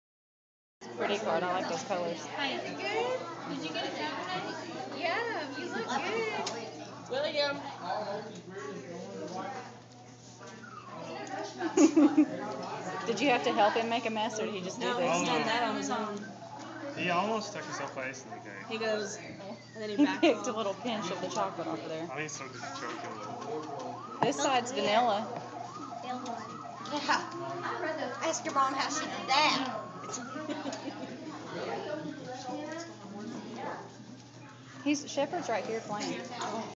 Happy first Birthday William!
My son eating his first birthday cake.